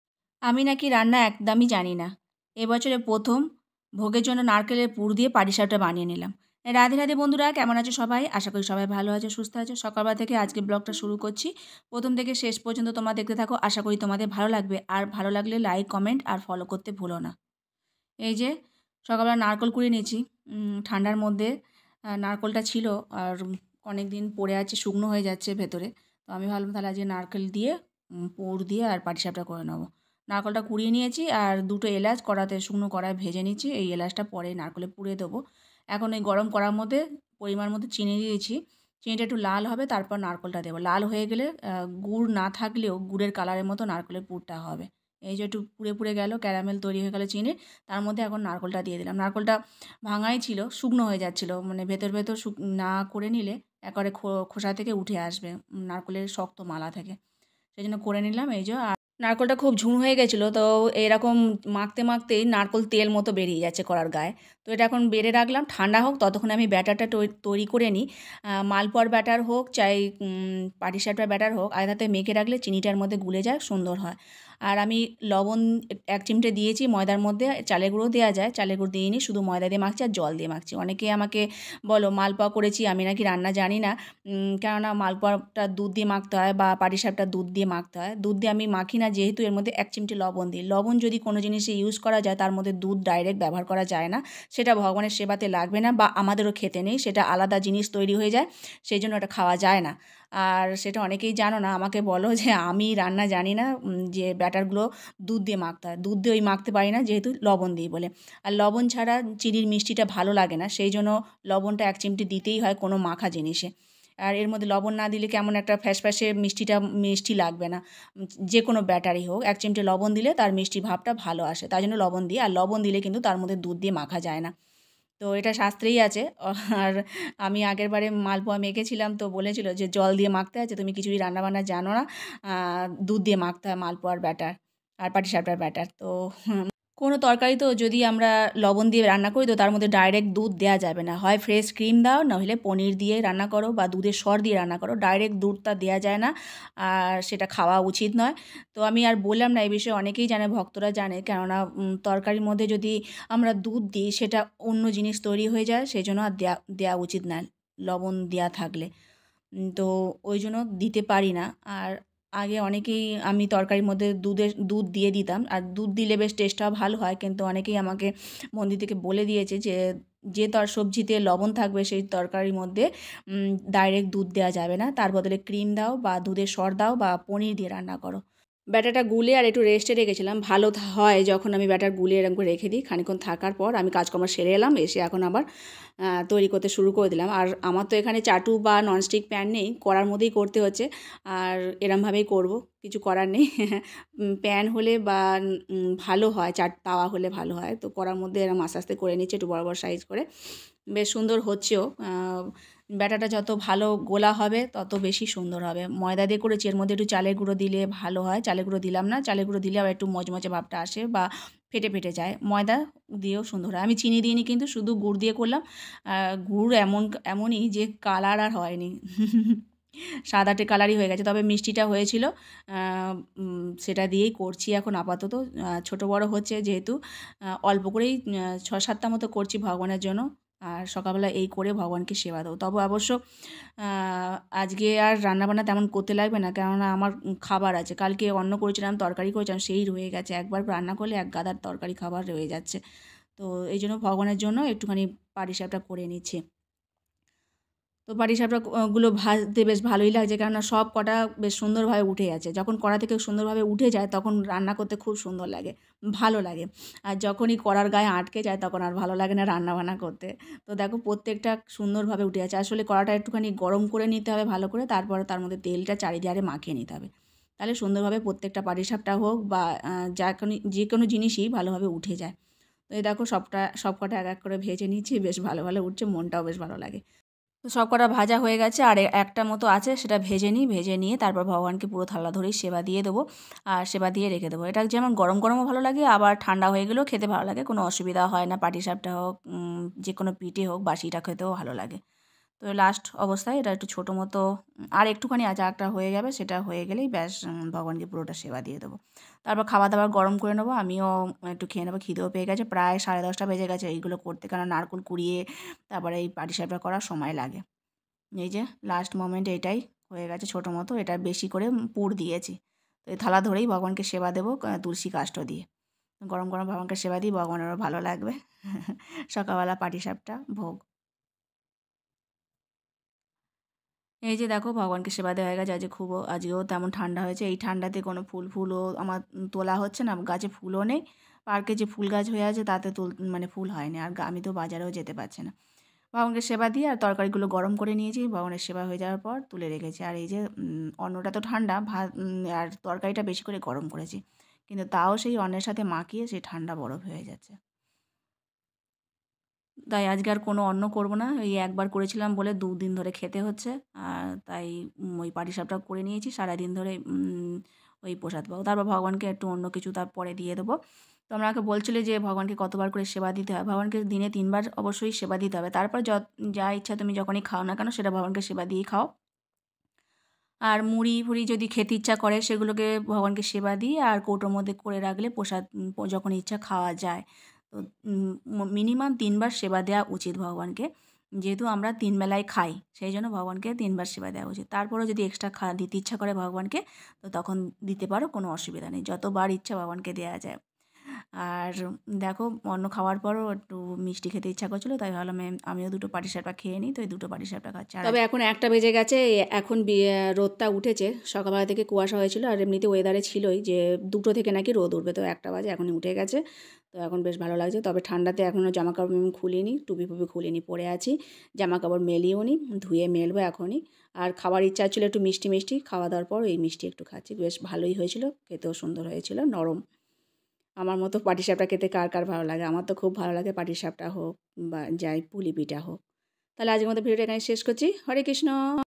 আমি নাকি রান্না একদমই জানি না/ভগবানকে দিনে কতবার ভোগ দিই/এই বছরে প্রথম বানালাম ভোগে জন্য নারকেলের প.
আমি নাকি রান্না একদমই জানি না/ভগবানকে দিনে কতবার ভোগ দিই/এই বছরে প্রথম বানালাম ভোগে জন্য নারকেলের পুর দিয়ে পাটিসাপটা।